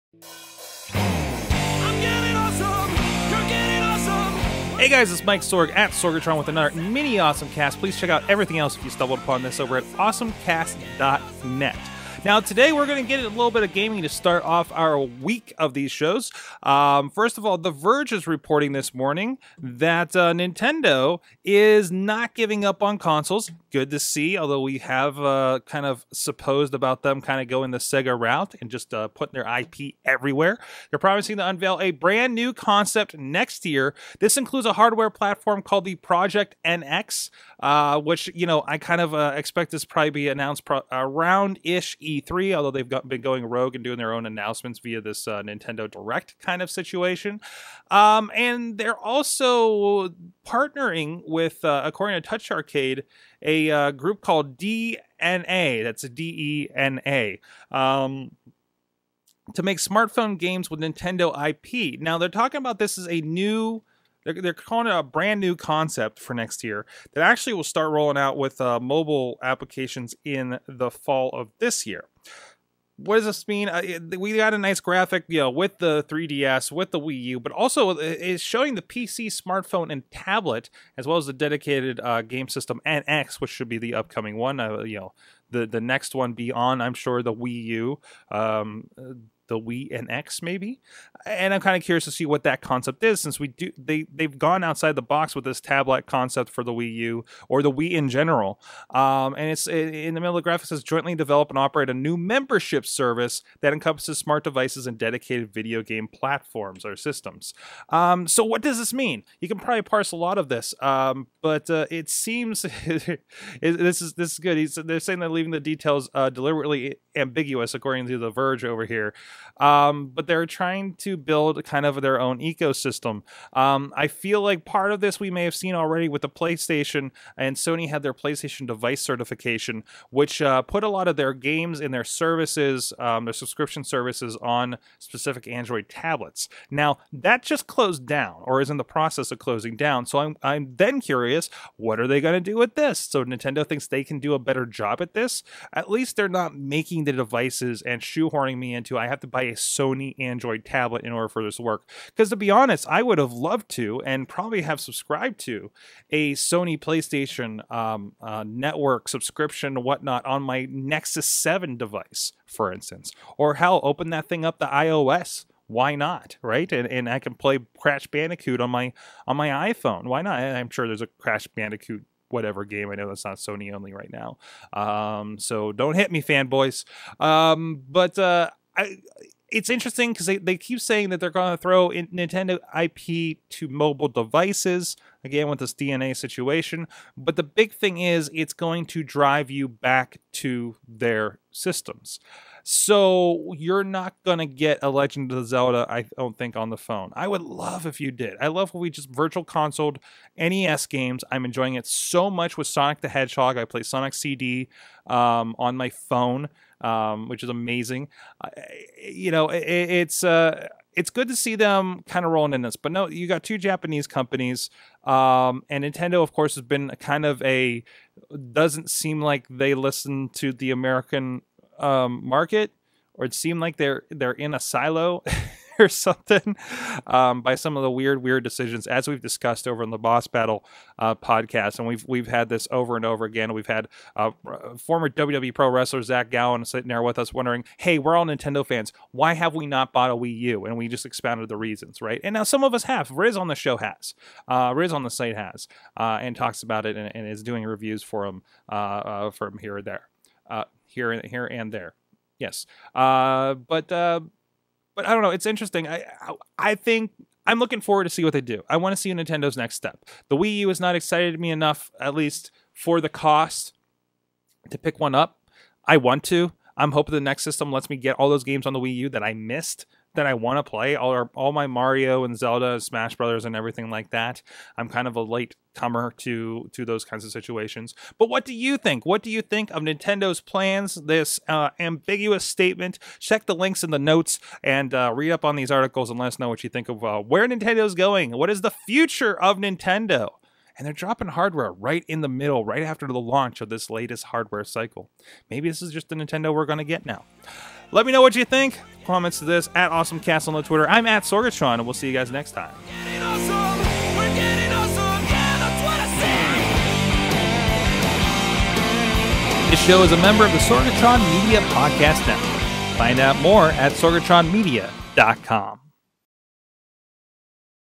Mini AwesomeCast: Nintendo's Next Step
Nintendo has announced the upcoming "NX" console is in the works, and an upcoming membership ecosystem encompassing their consoles and mobile devices with DeNA coming up. What could be in the works for Mario's parent company?

Source: Nintendo isn't giving up on consoles, promises to unveil a 'brand new concept' next year
Nintendo Partners with DeNA to Make Smartphone Games with Nintendo IP